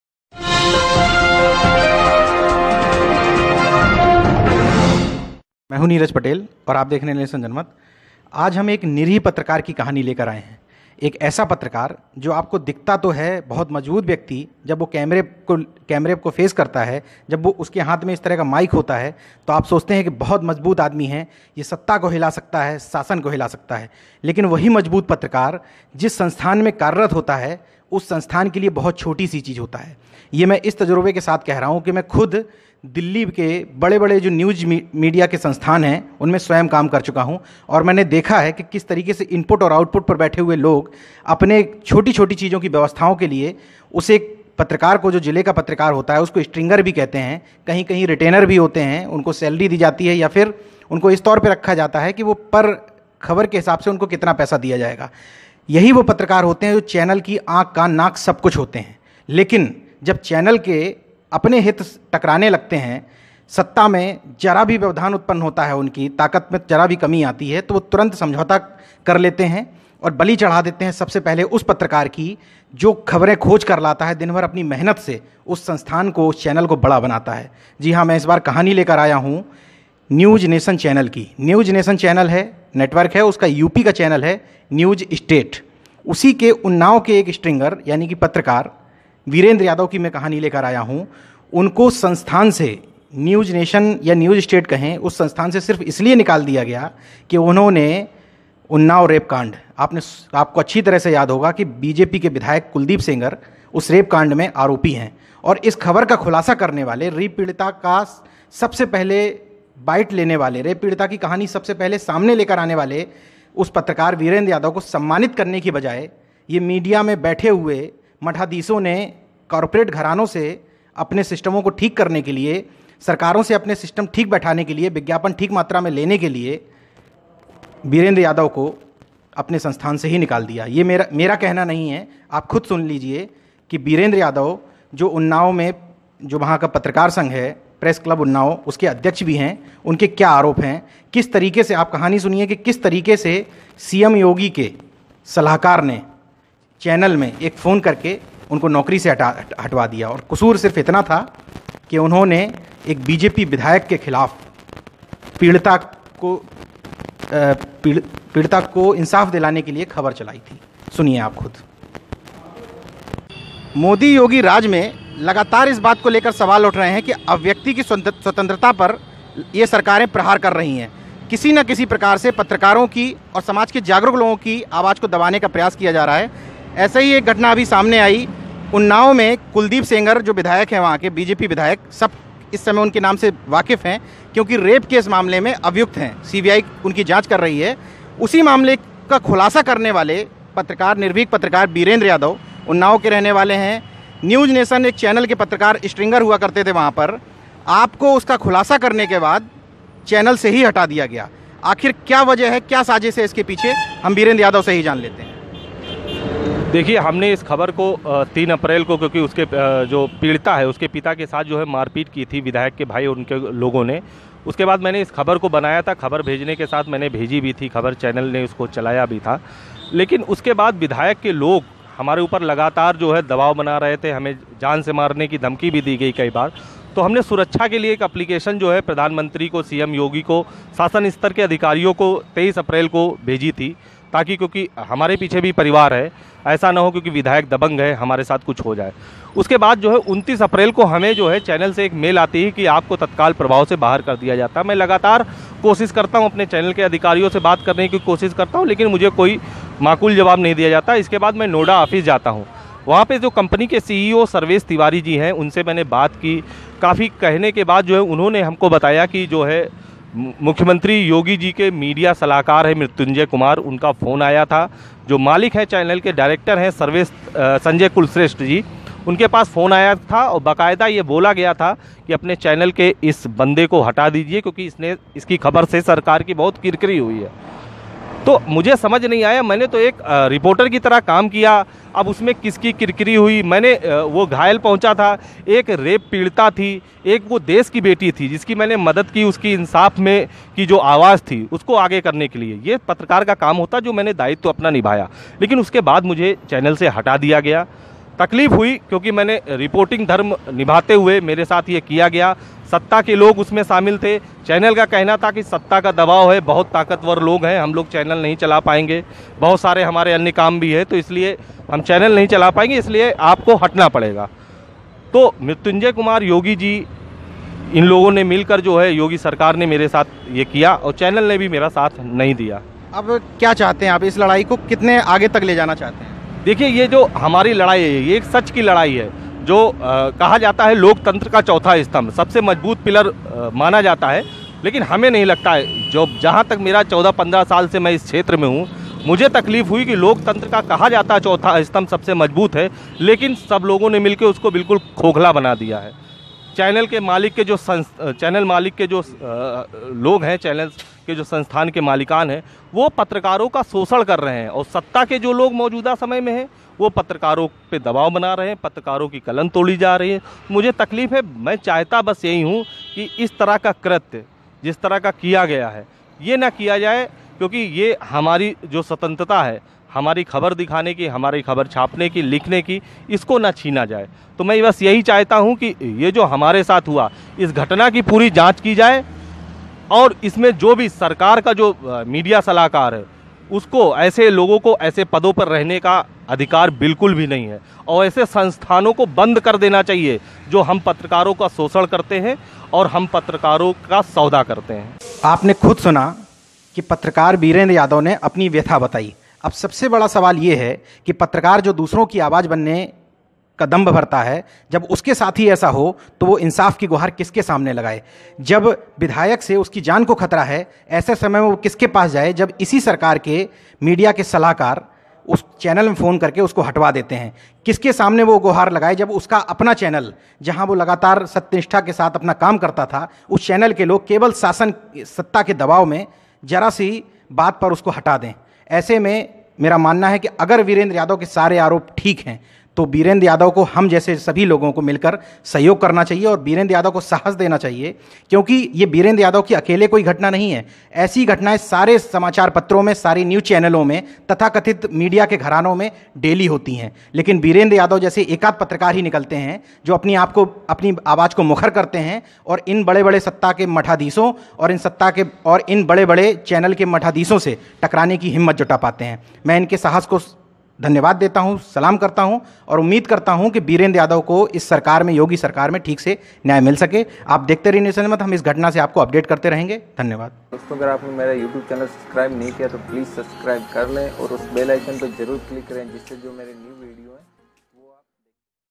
उन्नाव रेपकांड का खुलासा करने वाले पत्रकार वीरेन्द्र यादव को योगी सरकार के दवाब में चैनल से निकाला !
उन्नाव रेप कांड में आरोपी बीजेपी विधायक कुलदीप सेंगर के खिलाफ खबर चलाने वाले पत्रकार वीरेन्द्र यादव को सीएम योगी के मीडिया सलाहकार के इशारे पर नौकरी से हटा दिया न्यूज नेशन चैनल वालों ने !